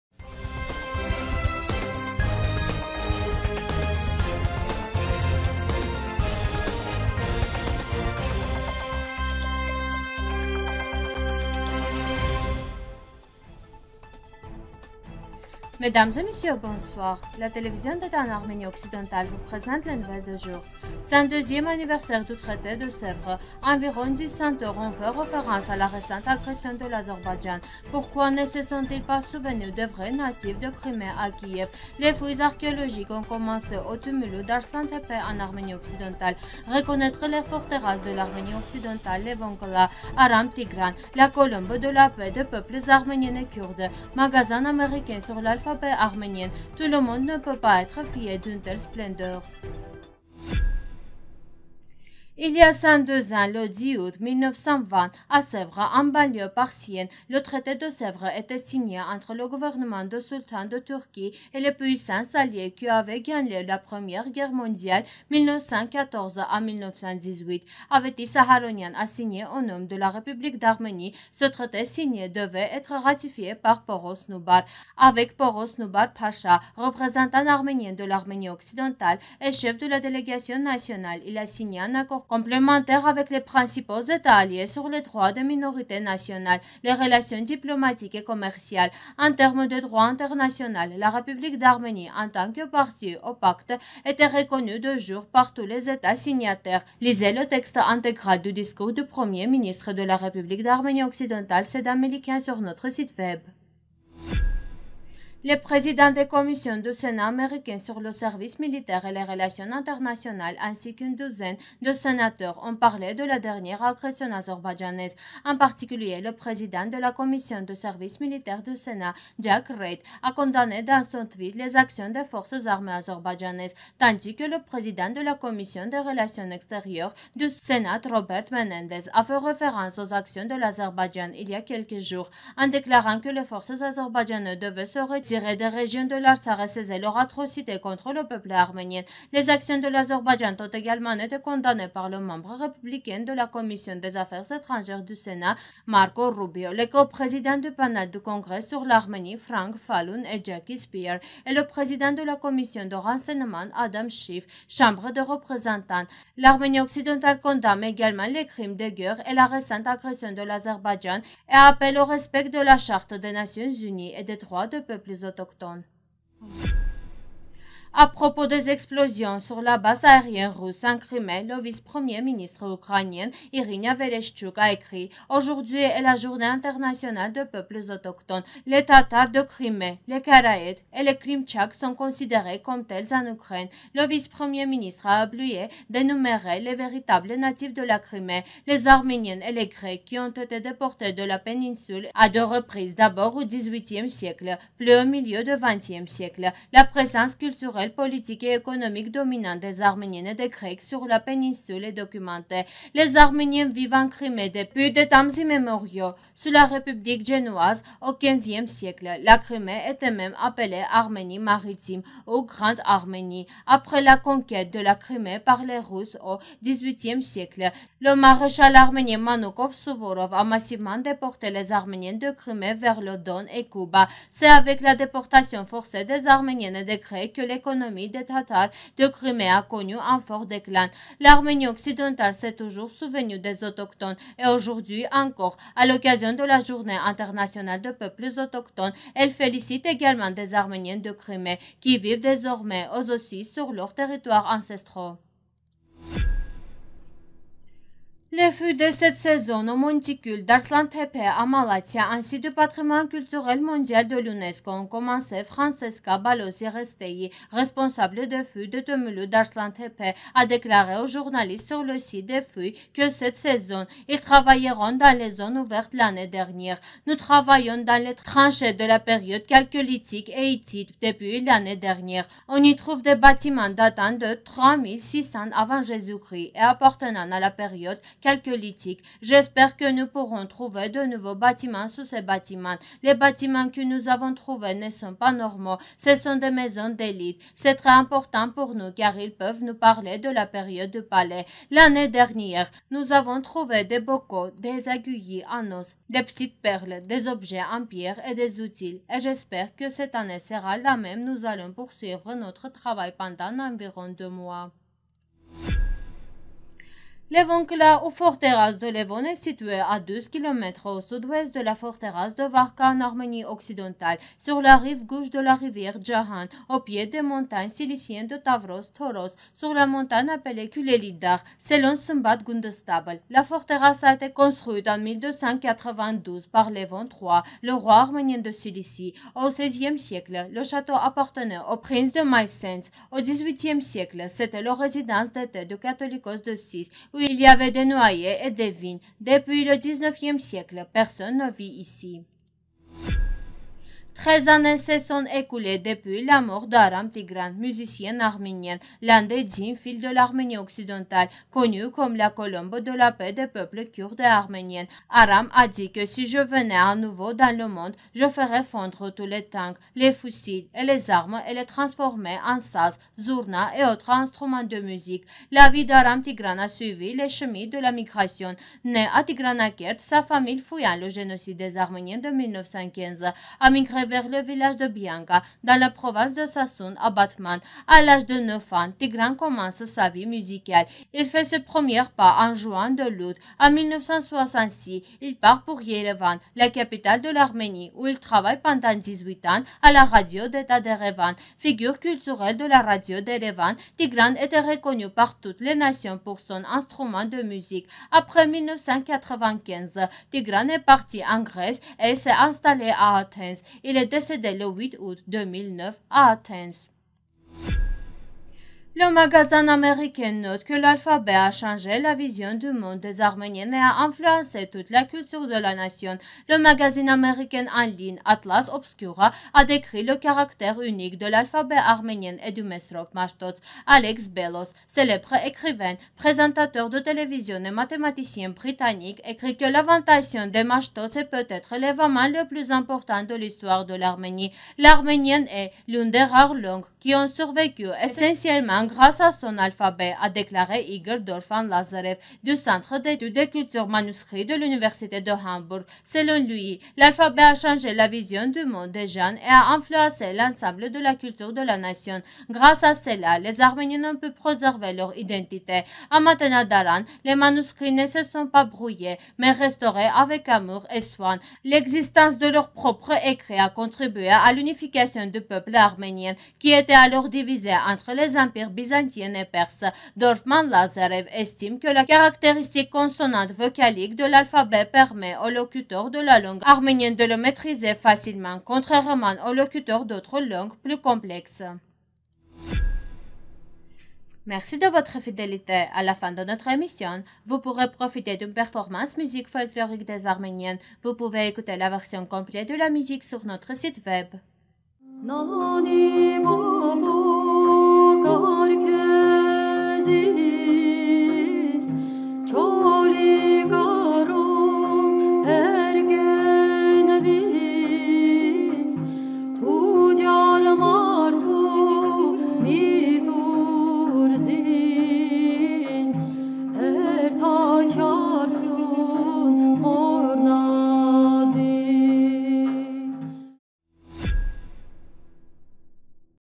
Les fouilles archéologiques ont commencé au tumulus d'Arslantepe en Arménie Occidentale
1. Cent-deuxième anniversaire du traité de Sèvres
2. Environ 10 sénateurs ont fait référence à la récente agression de l'Azerbaïdjan.
3. Pourquoi ne se sont-ils pas souvenus des vrais natifs de Crimée à Kiev ?
4. Les fouilles archéologiques ont commencé au tumulus d'Arslantepe en Arménie Occidentale.
5. Reconnaître les forteresses de l'Arménie Occidentale : Levonkla
6. Aram Tigran, la colombe de la paix des peuples arménien et kurde.
7. Magazine américain sur l'alphabet arménien : "Tout le monde ne peut pas être fier d'une telle splendeur"